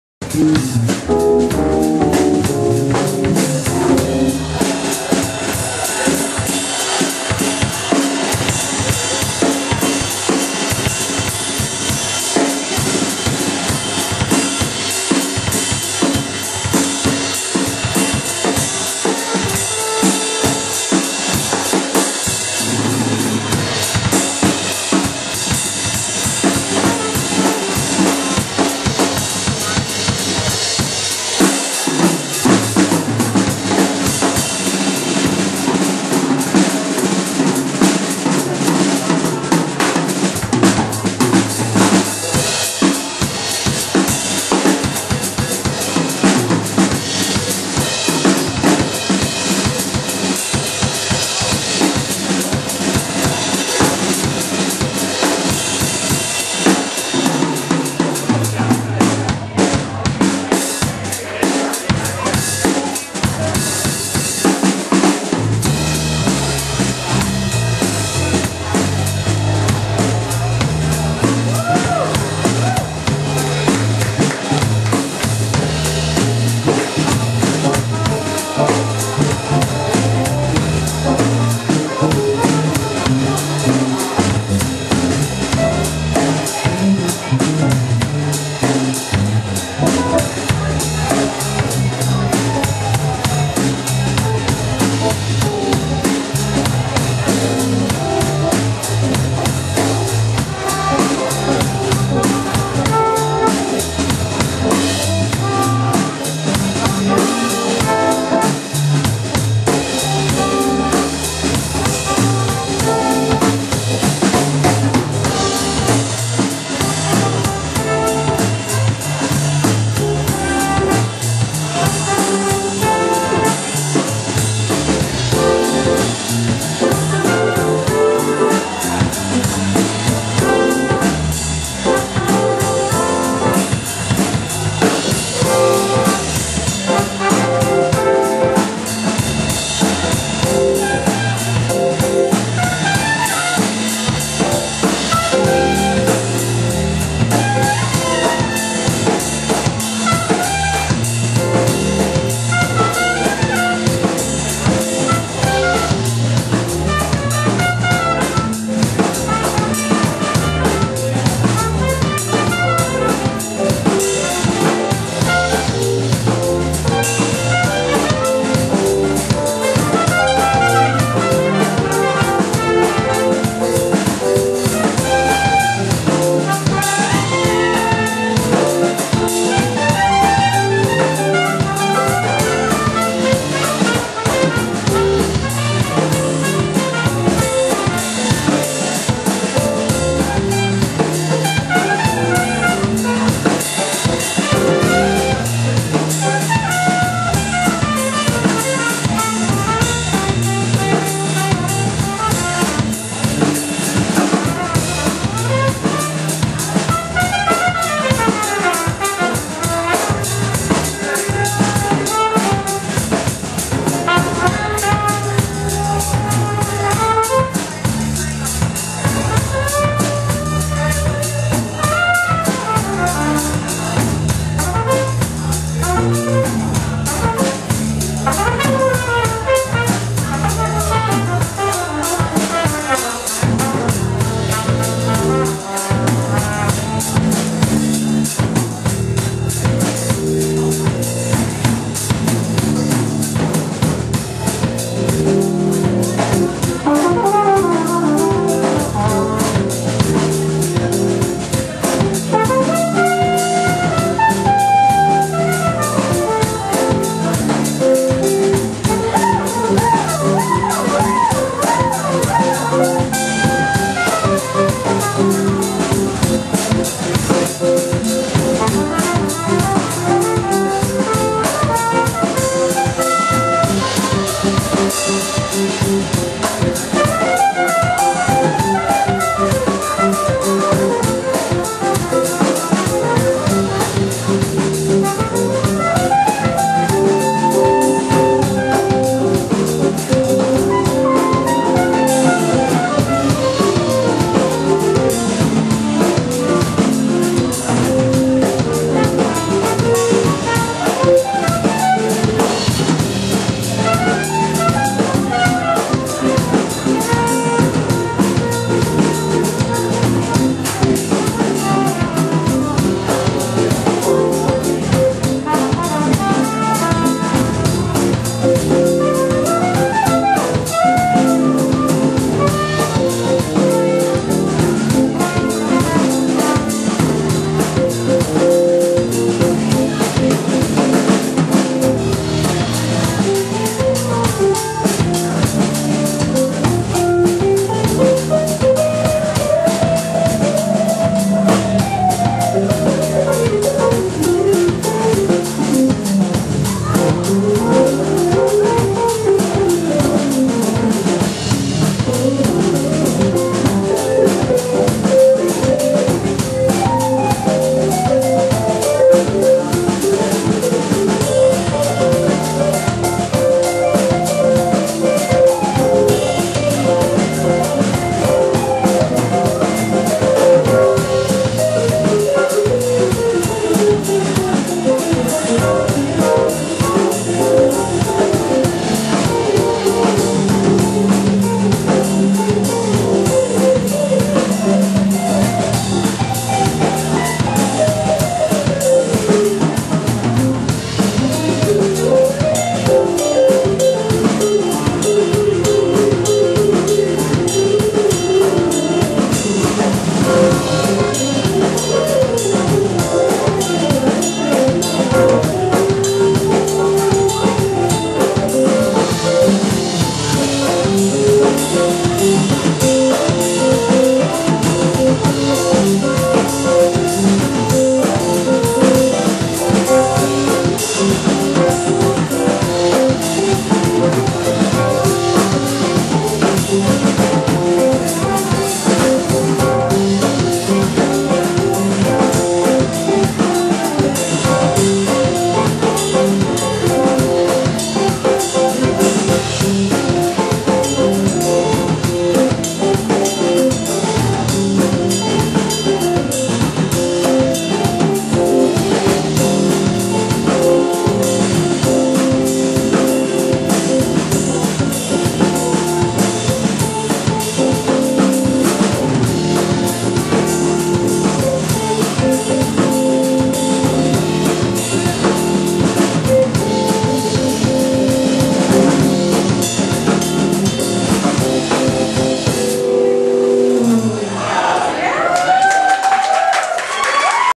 FunkShua Latin Groove Part 2
FunkShua Jam at Chicora Alley Greenville, SC July 12th 2012 Latin Jam Part 2